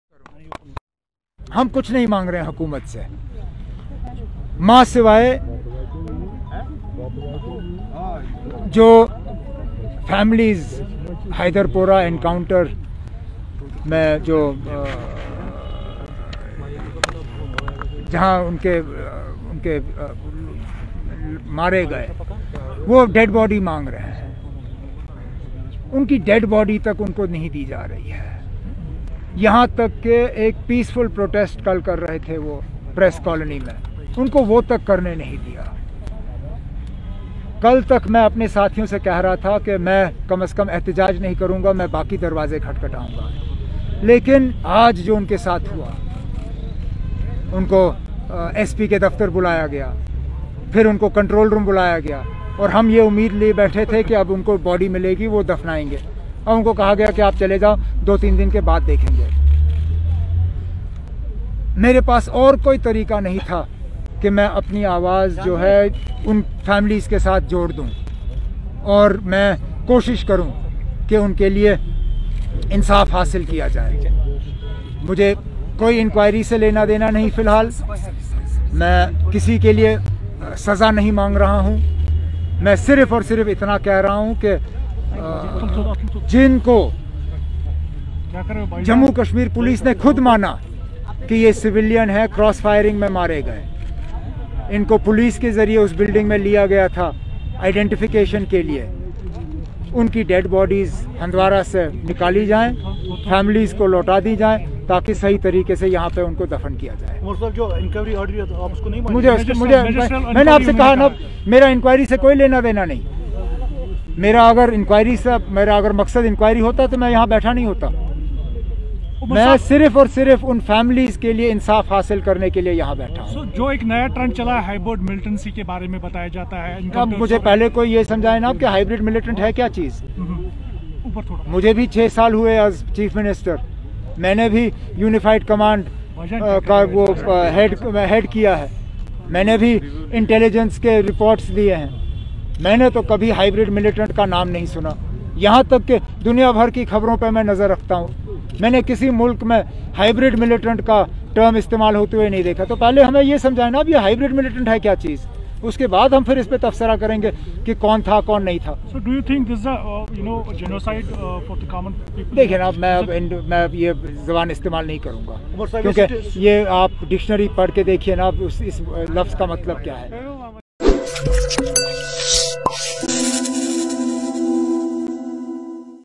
Omar on sit-in seeking Hyderpora victims' mortal remains to be returned
Former Jammu and Kashmir Chief Minister Omar Abdullah staged a sit-in protest in Srinagar on Thursday in support of families seeking return of the mortal remains of those killed in Hyderpora during a gunfight on Monday.